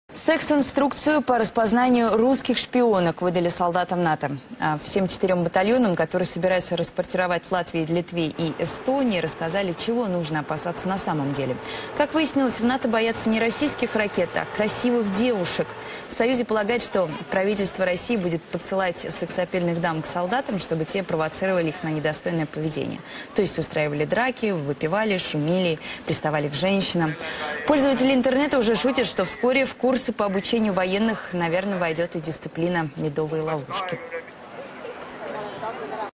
Солдатам НАТО раздали секс инструкцию по распознаванию шпионок из РФ
Организация боится, что Россия будет в качестве провокации подсылать к военным, расквартированным в Прибалтике, красивых девушек.